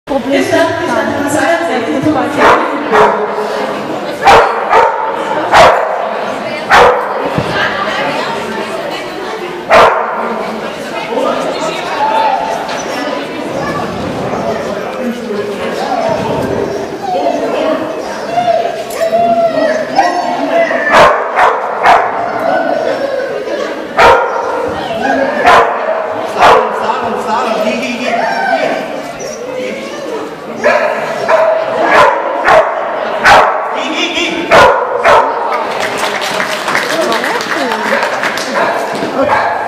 Thomas Ebeling mit Wumm, Ammersee, 14.01.2012, Jumping
Sieg im Jumping!